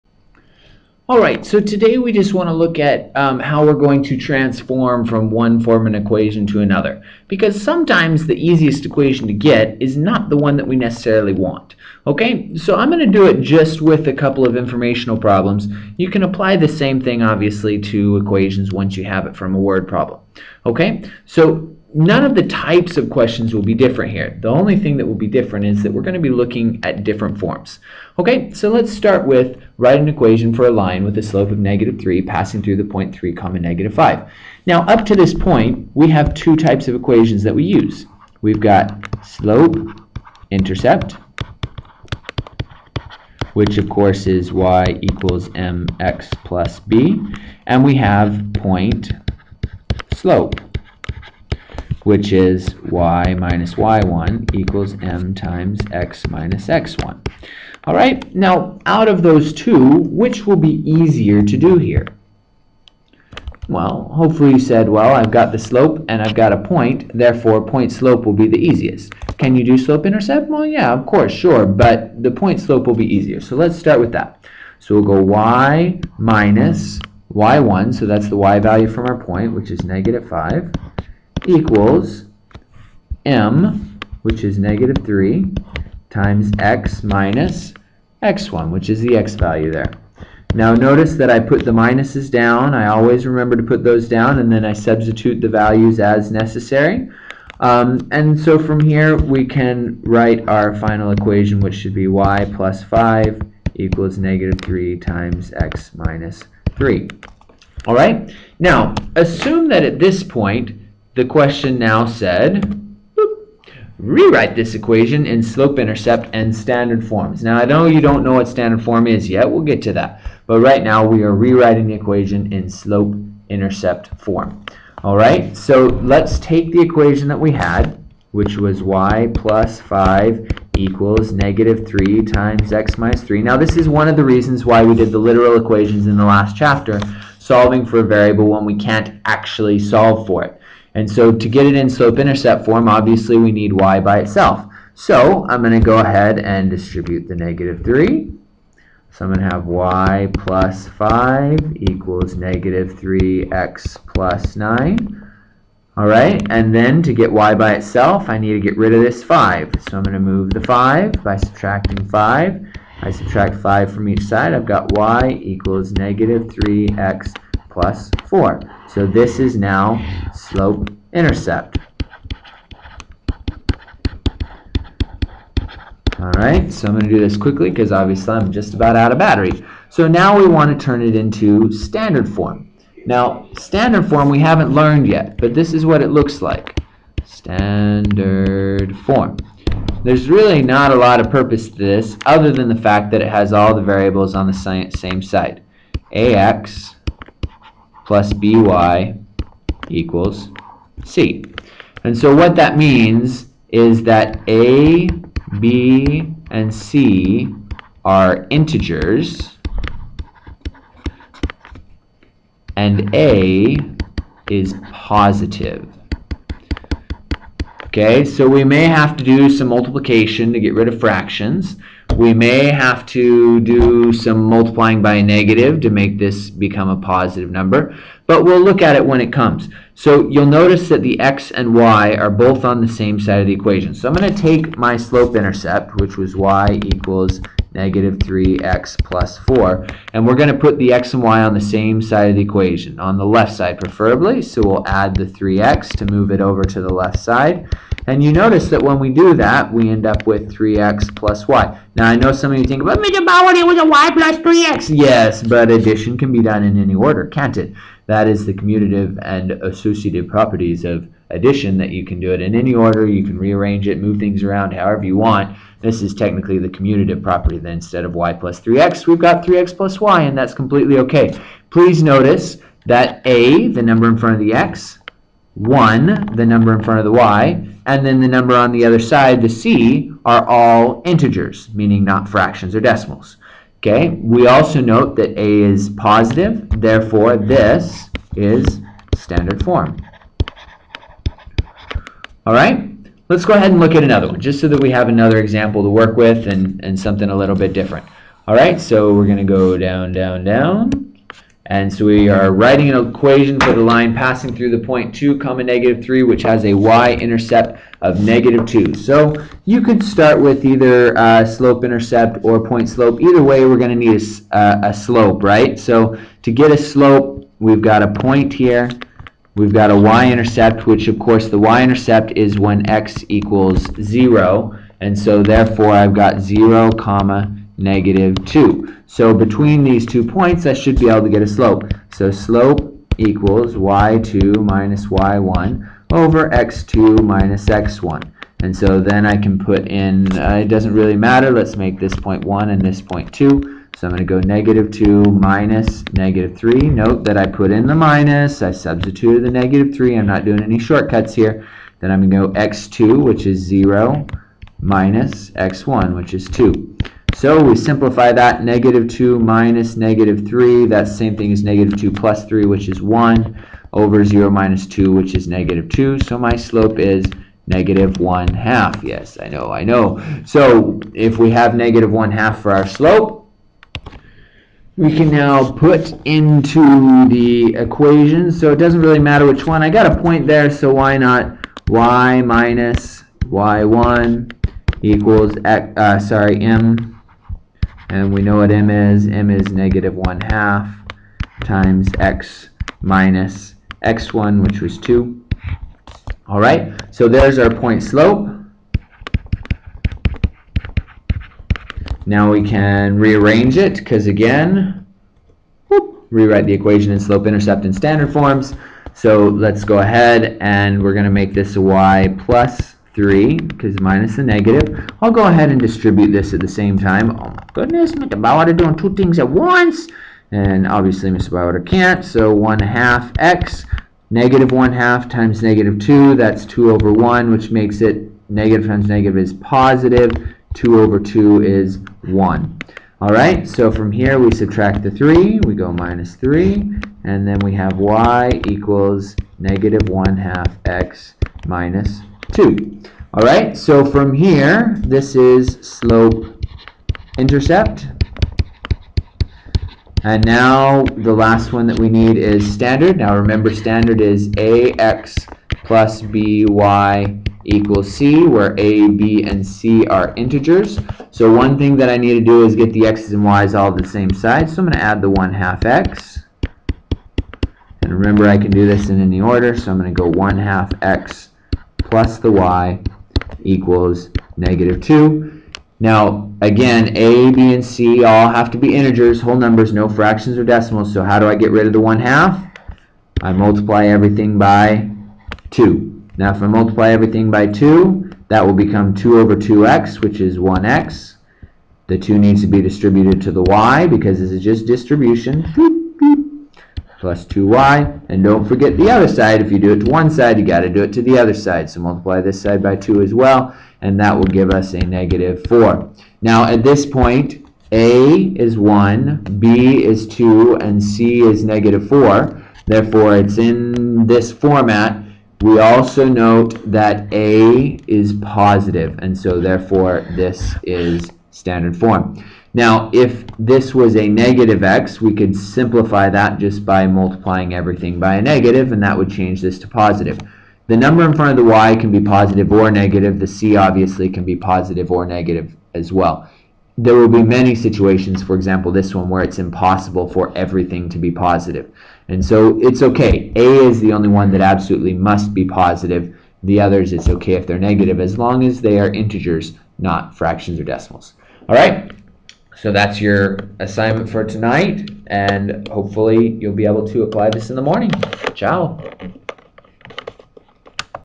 Rewriting Linear Equations
How to convert point-slope to slope-intercept to standard forms of a linear equation